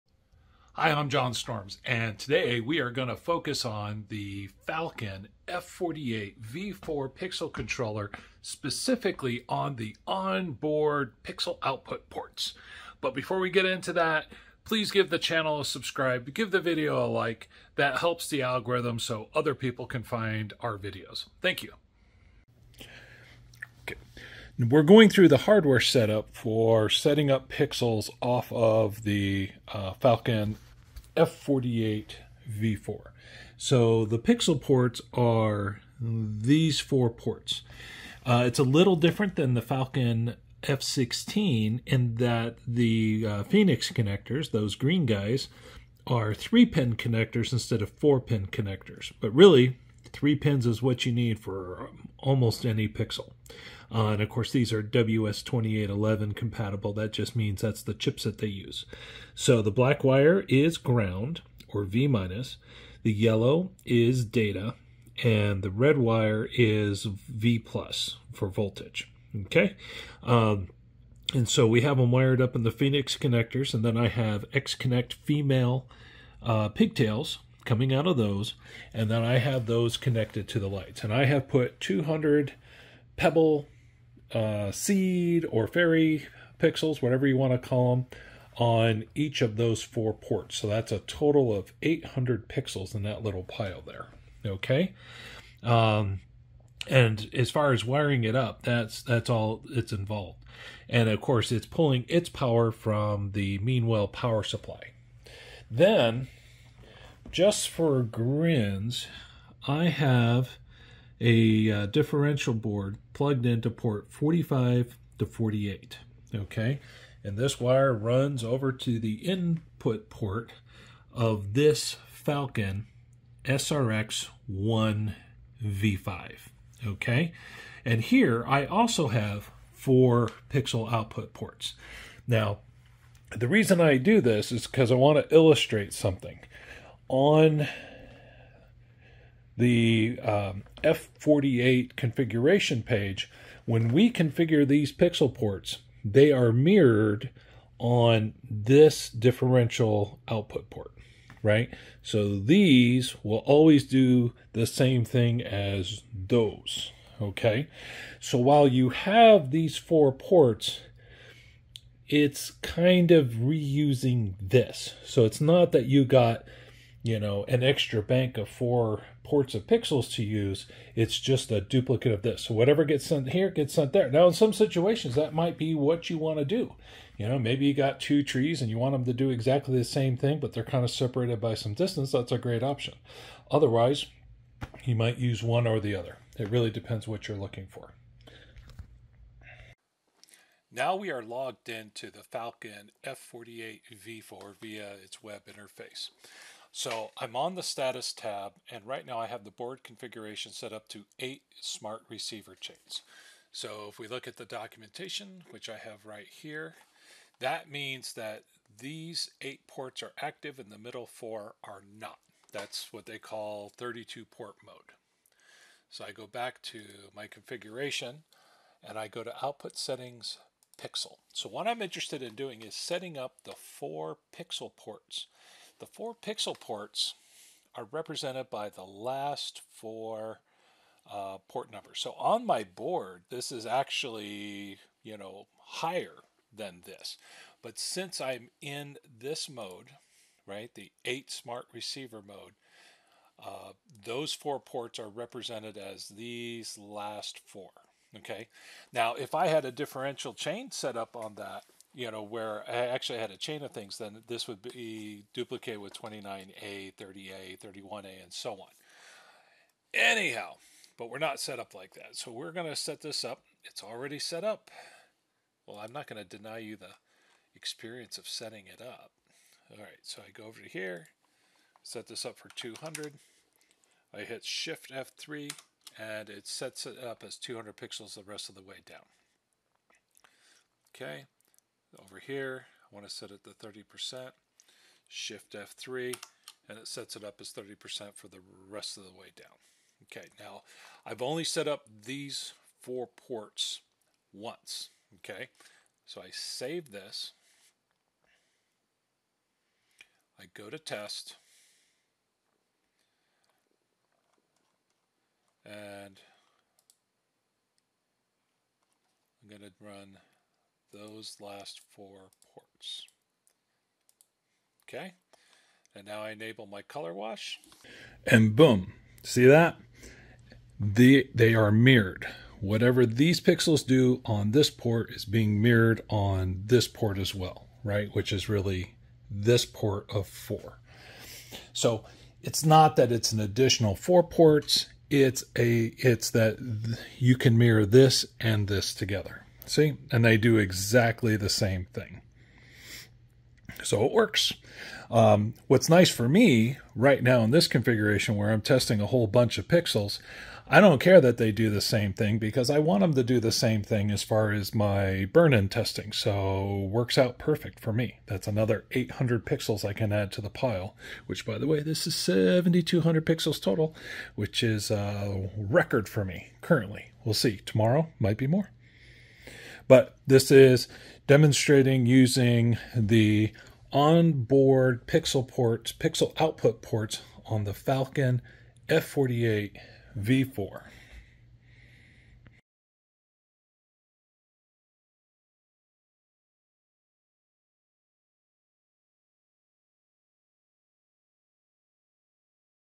Using On Board Pixel Ports on the Falcon F48v4
A big difference between the Falcon F48v3 and v4 is the addition of 4 onboard pixel output ports on the F48 board itself. It does have a catch those that the 4 pixel ports are mirrored on the last differential port.

Falcon F48v4 pixel controller
Falcon SRx1 smart receiver board